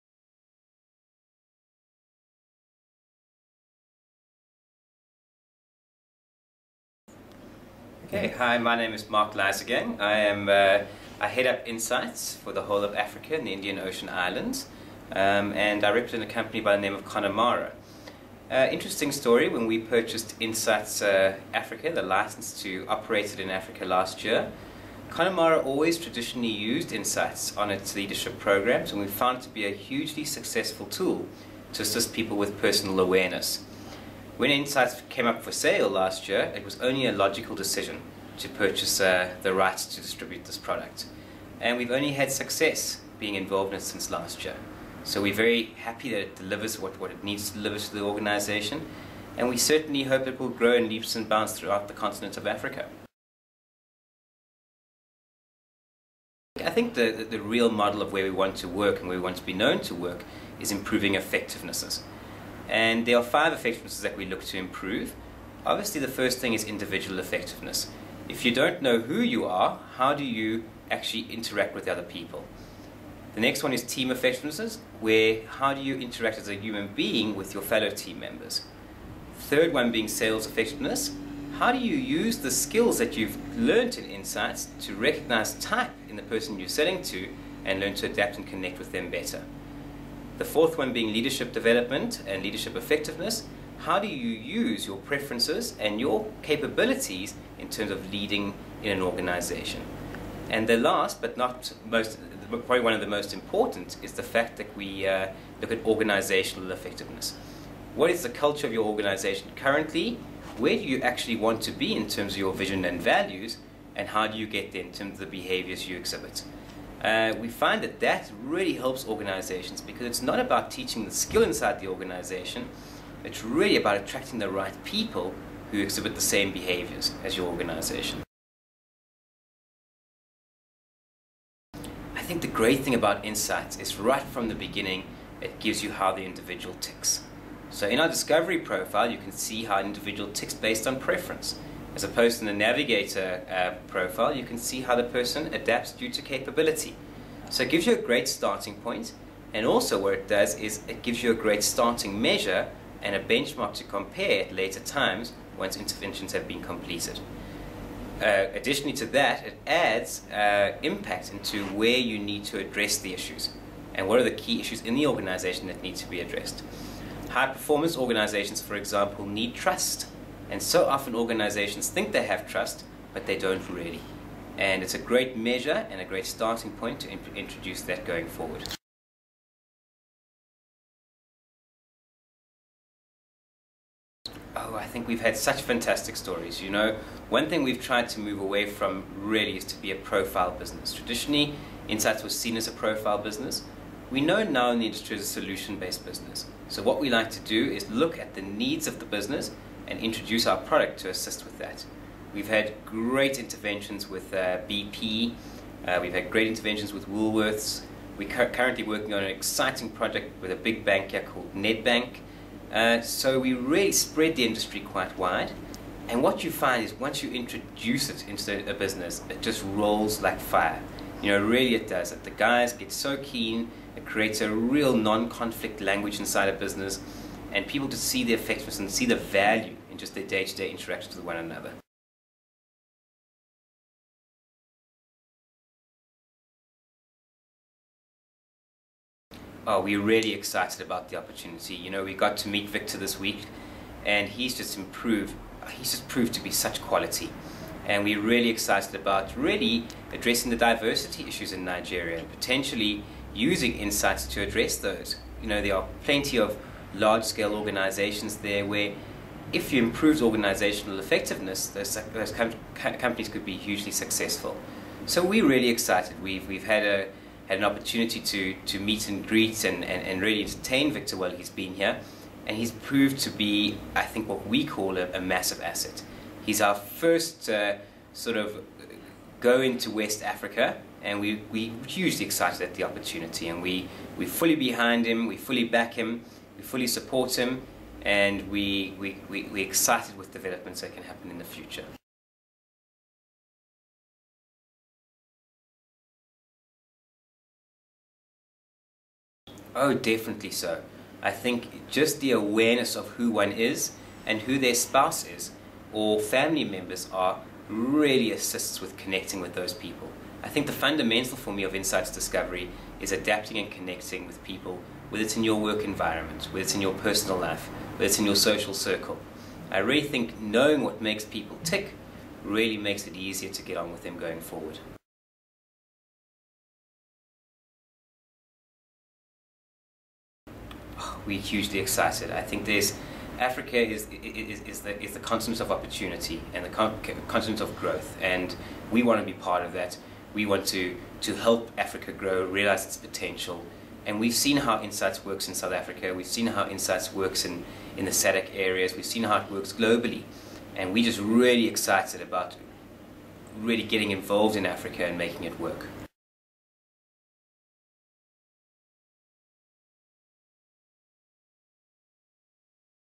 Insights in Africa
Mark Leisegang, Head of Insights Team at Connemara South Africa discusses Insights Discovery and the future of Insights in Africa